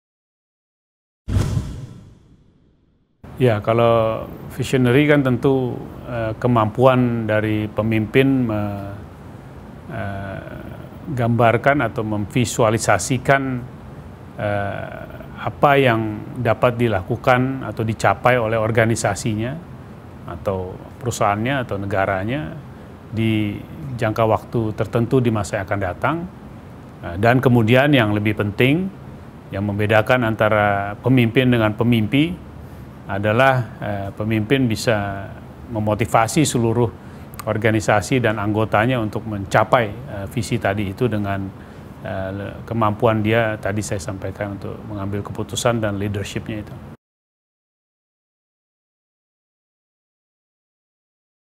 50 Second Inspiration: Visionary - Mahendra Siregar, Kepala Badan Koordinasi Penanaman Modal RI
(me (member of Vibiz Media Network)
Program : Tea Break by Businesslounge.co
Sub Program : 50 Second Inspiration

Mahendra Siregar menggambarkan visionary sebagai kemampuan untuk menvisualisasikan apa yang dapat dicapai oleh organisasinya pada masa yang akan datang dan bagaimana membawa seluruh anggotanya untuk mencapai visinya.

Diproduksi 13 Agustus 2014

Simak berita dan tayangan inspiratif selengkapnya mengenai para pemimpin perusahaan atau CEO terkemuka lainnya di